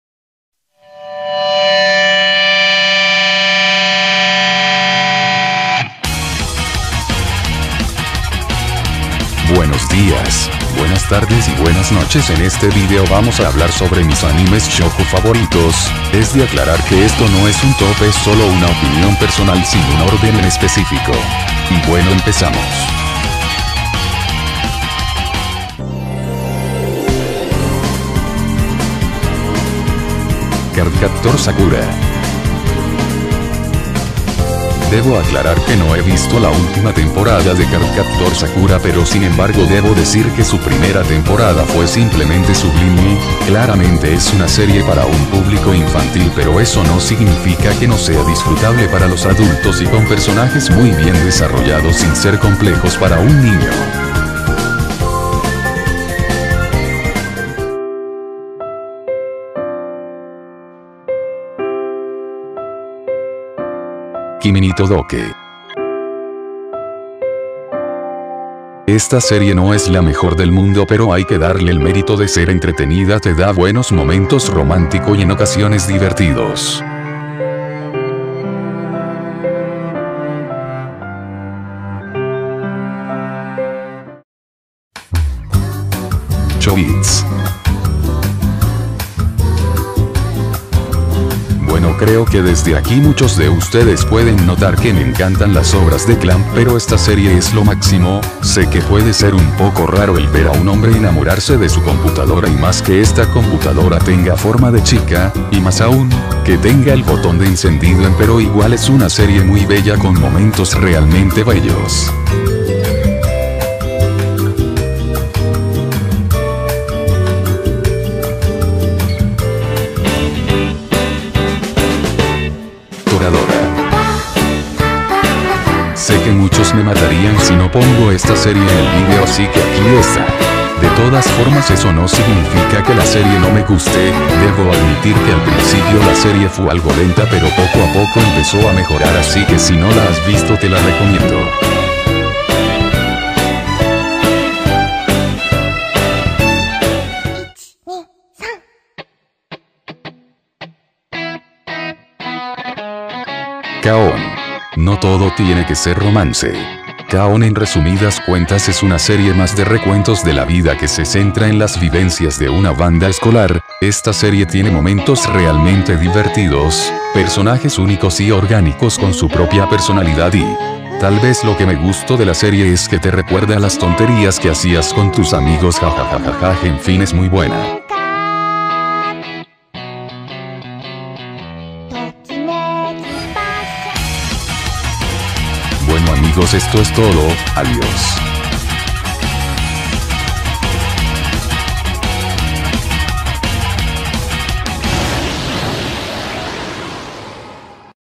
mis 5 animes shojo favoritos.
Este vídeo es una lista personal de mis animes shojo favoritos.